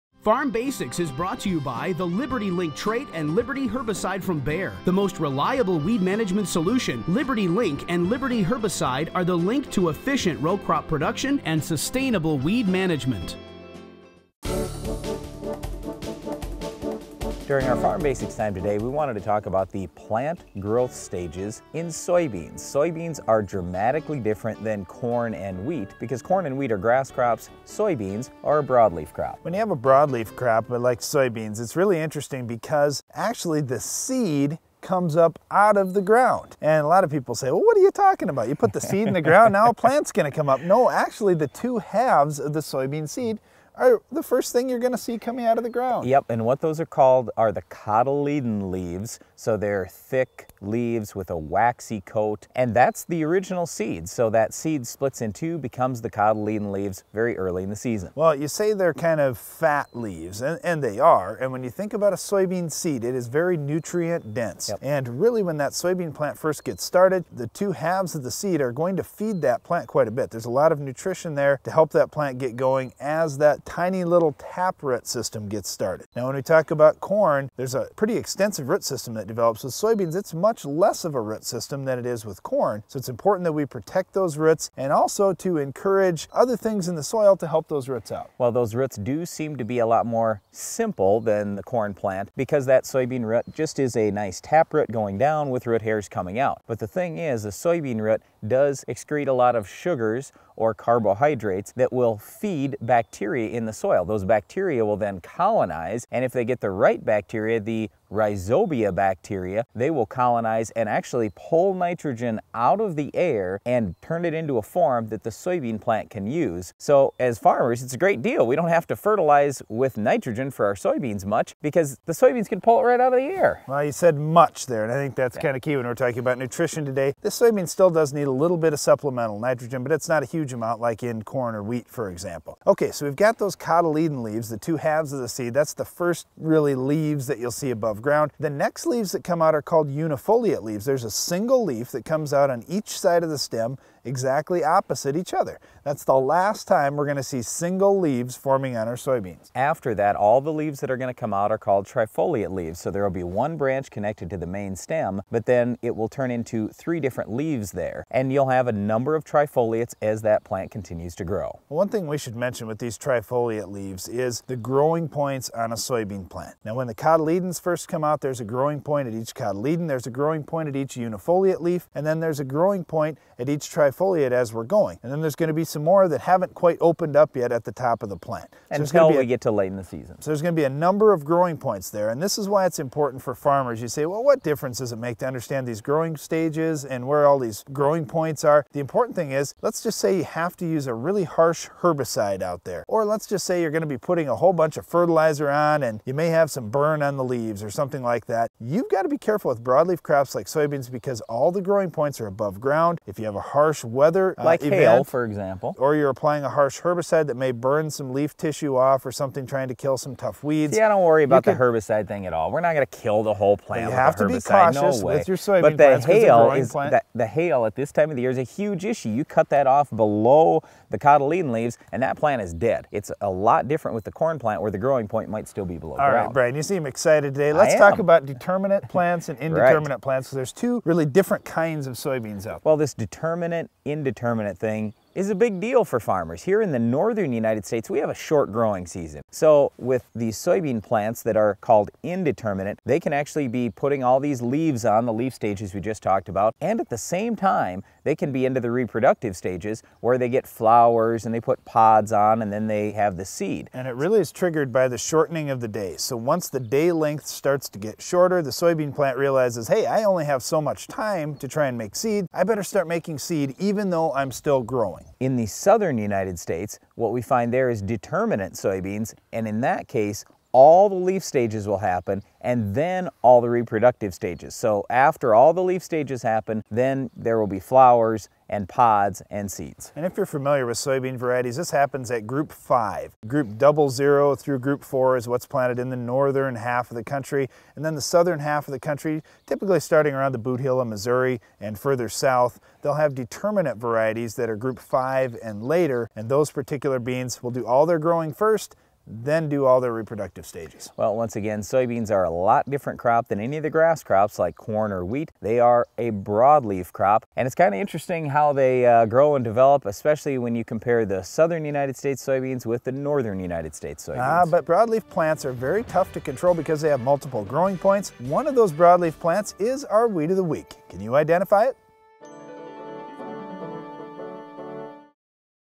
Farm Basics #738-Soybean Growth Stages (Air Date 5/27/12)
Brian & Darren go over the various growth stages of soybeans.